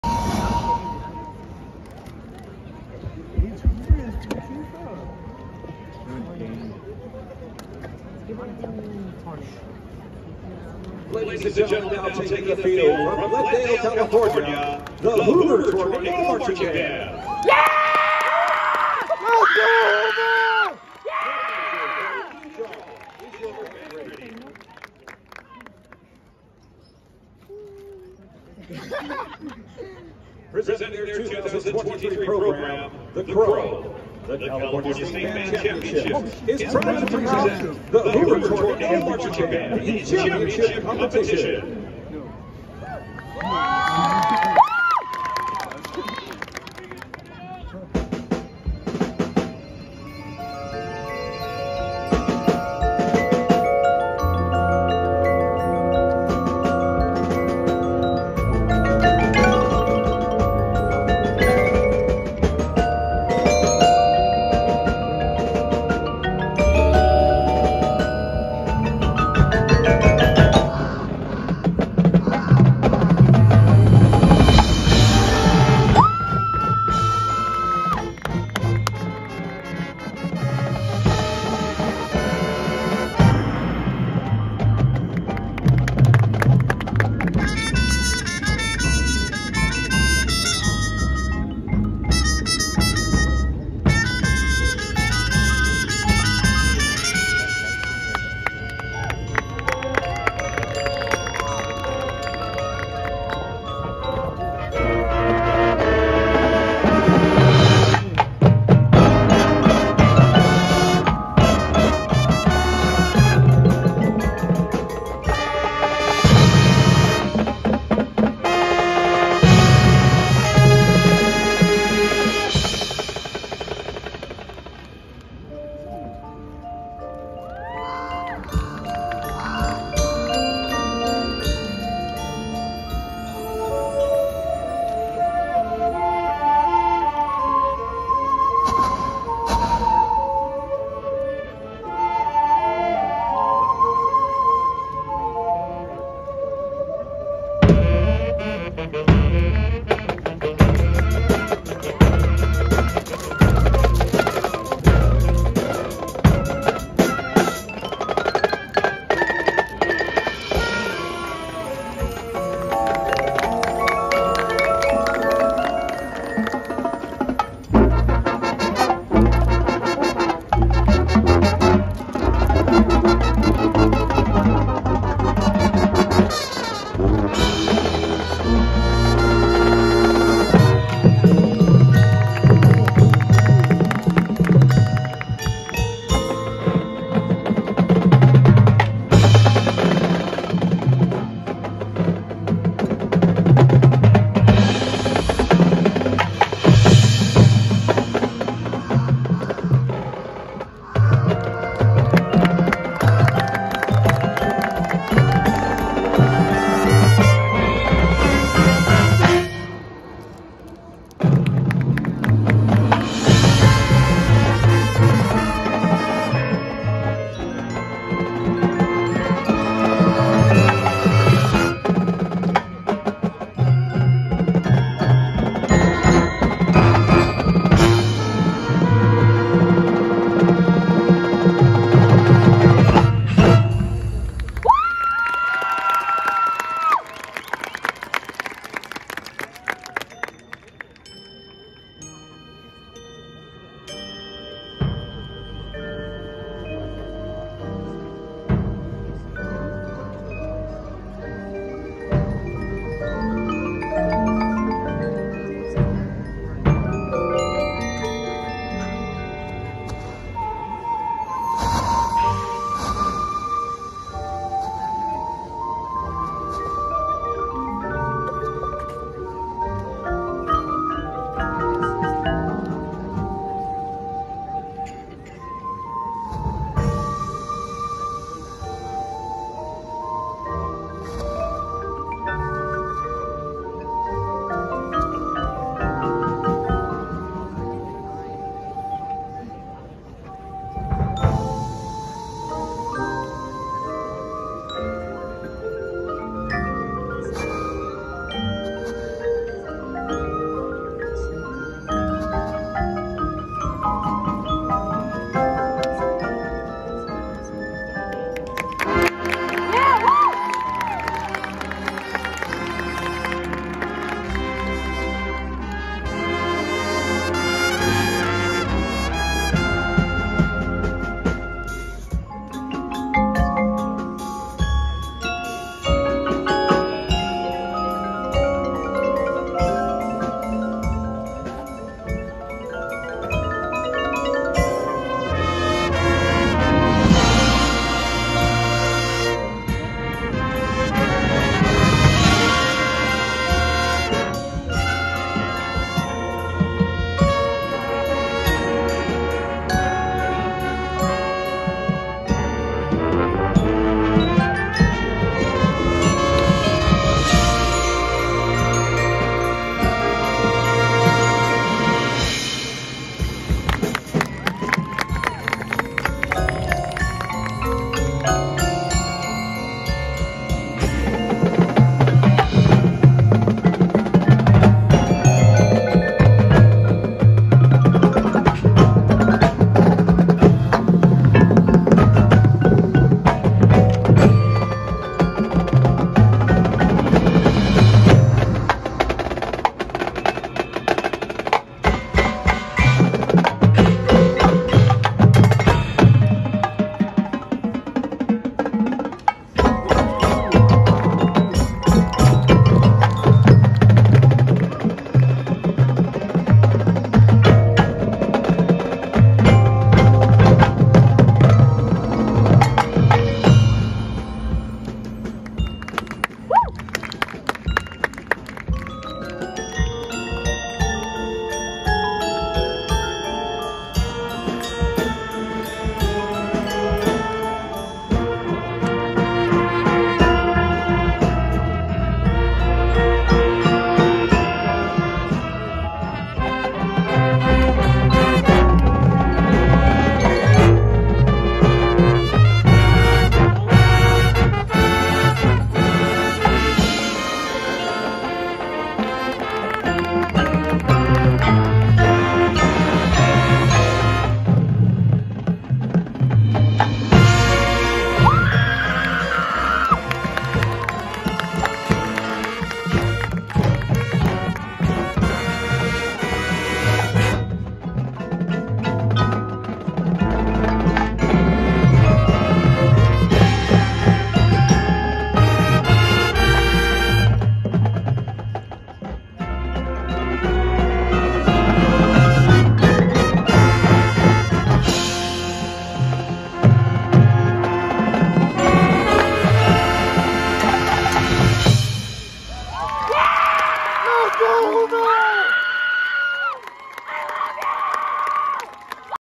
Herbert Hoover High School - The Hoover Tornado Marching Band (Newport Harbor 2023)
2023 Southern California Division 2 Championships
Saturday, November 11, 2023 - Newport Harbor High School